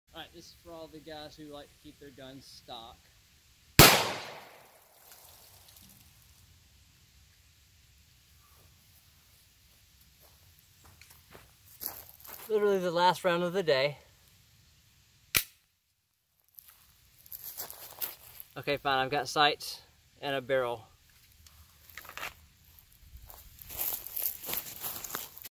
Stock Glock Range Tricks
Just shooting weeds with the glock. Stock trigger. I did upgrade the sights and barrel.